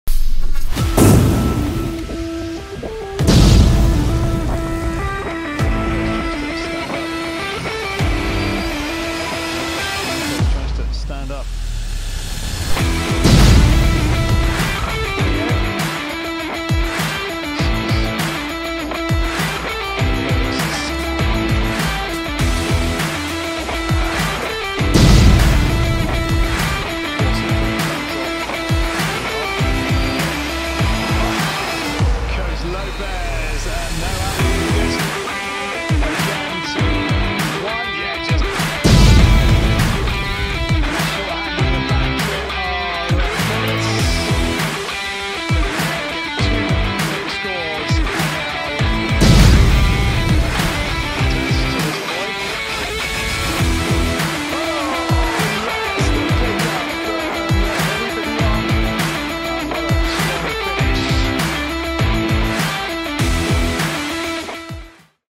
TOP 5 vs Umana Reyer Venecia (GrancaTV, 07.04.22)
Llega un TOP con claro sabor a La Vega. Javi, Olek y Khalifa, protagonistas del último partido de grupos en EuroCup.